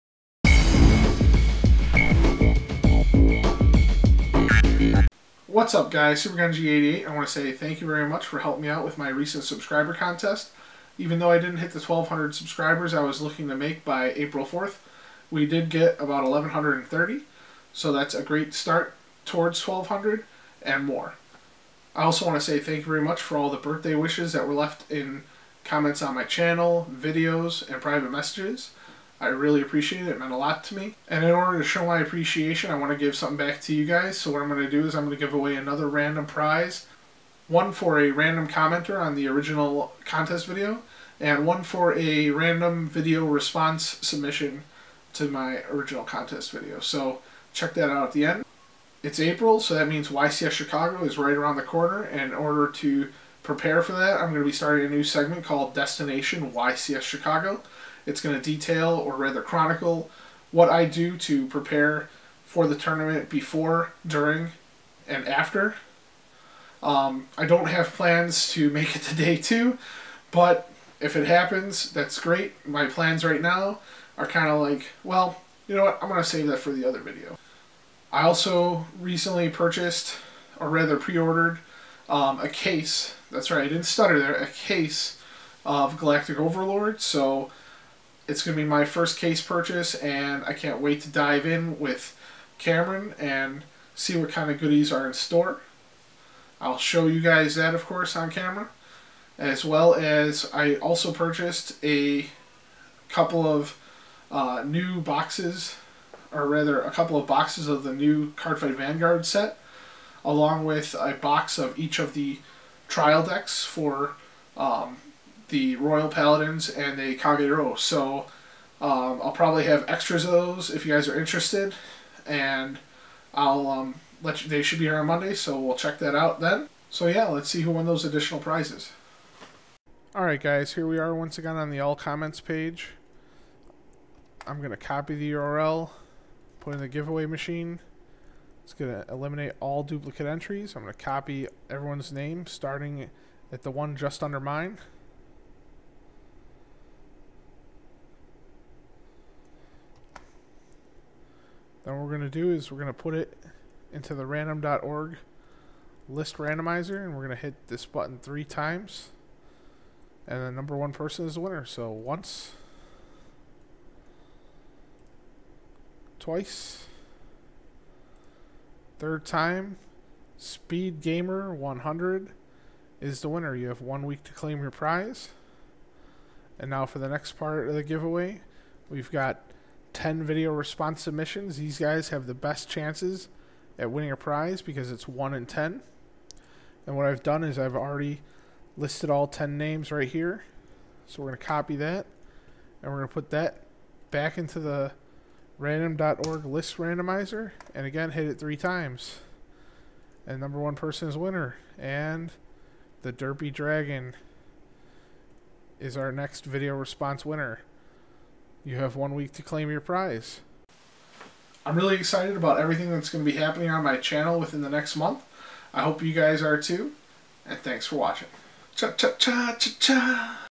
~ Yugioh ~ SG88 Channel Update 4/6/2012
The Great Yugioh Giveaway Contest has ended and here is an update regarding that as well as the other things that will be happening on my channel during the month of April.